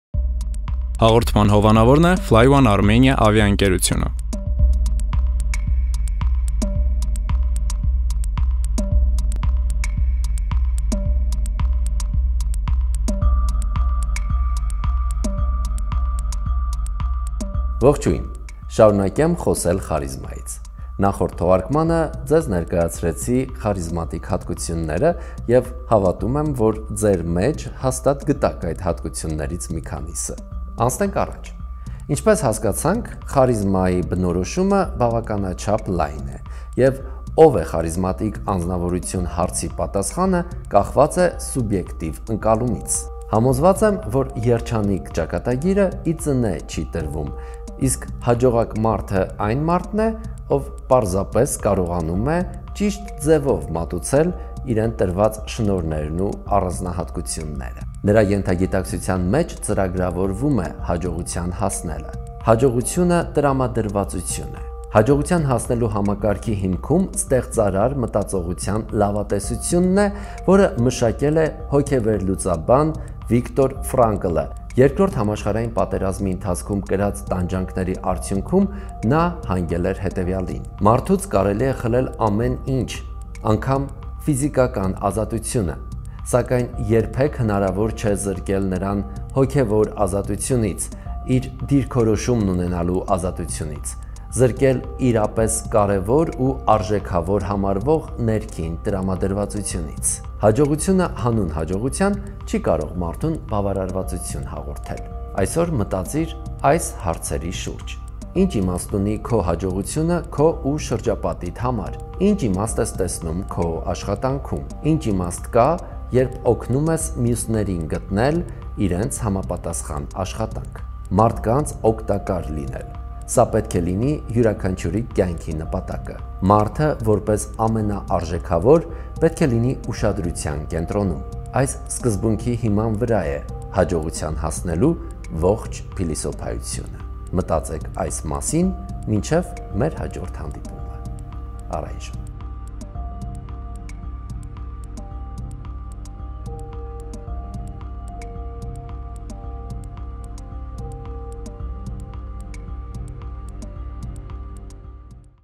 Թիվ 1 Խարիզմա | Հաջողության փիլիսոփայությունը
📌 Որտե՞ղ է սկսում հաջողությունը:

📌 Ինչպե՞ս են մտածում հաջողակները:

📌 Խարիզման և հաջողությունը: